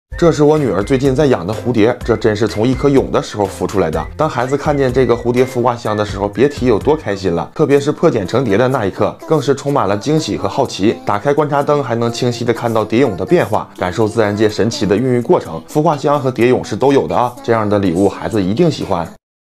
hatching butterfly observing insects
Children will be surprised to see the gift, witness the process of butterfly cocoon breaking, and feel the wonder of life.